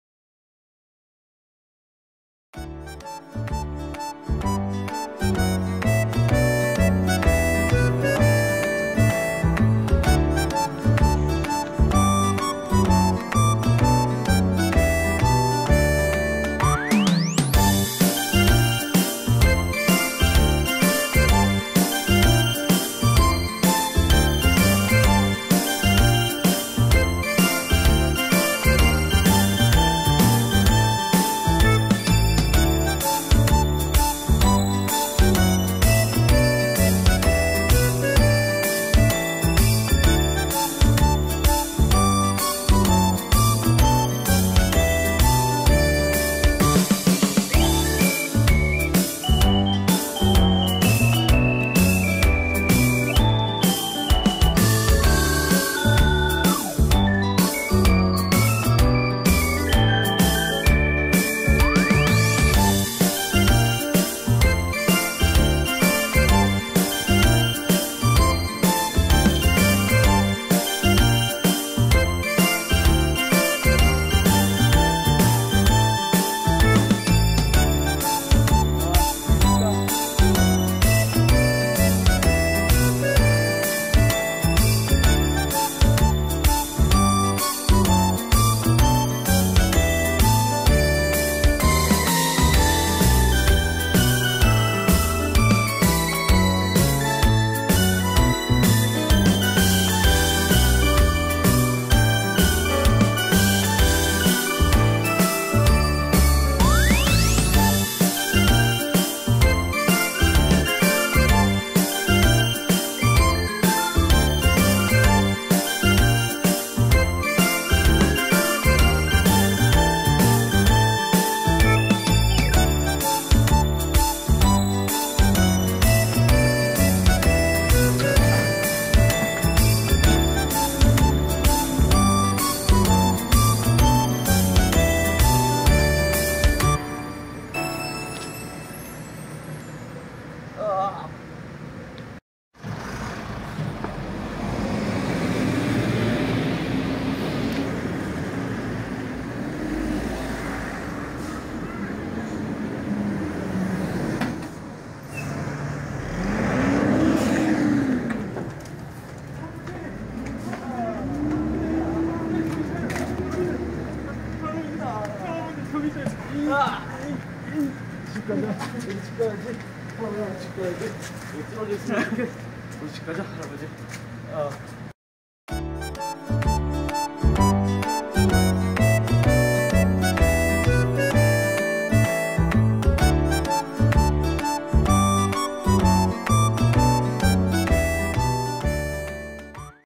노인 보호 스마트 조끼
노인 보호를 위한 스마트 조끼

조끼에 라즈베리 파이와 카메라가 들어갔습니다!

노인분의 위치를 찾는 것 뿐만 아니라 커뮤니티 기능까지 포함!
이제 노인분들께서도 손쉽게 SNS를 접할 수 있습니다.

센서를 이용하여 노인분이 쓰러졌을 때 알림!
또한
어려운 기계 사용에서 벗어나 단추 하나 누르는 것으로 행복한 교류를 즐길 수 있습니다!

[행복한 과학 기술 공모전 출품작]

제작
박수한, 박찬호, 김지훈, 박휘재

라즈베리파이, 웨어러블, 노인 보호, 노인 복지